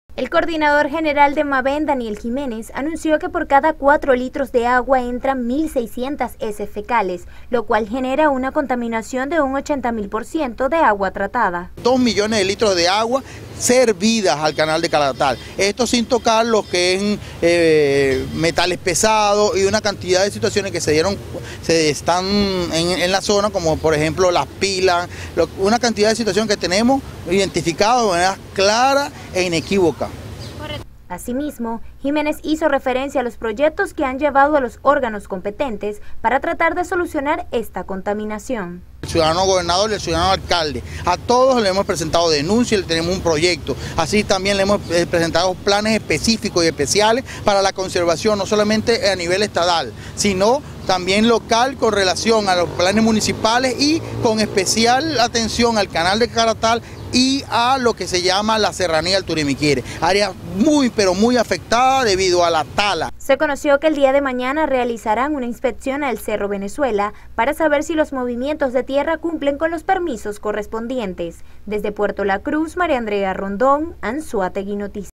Diariamente caen al canal de Caratal dos mil litros de aguas servidas
ANZOÁTEGUI NOTICIAS
REPORTE DE MARÍA ANDREA RONDÓN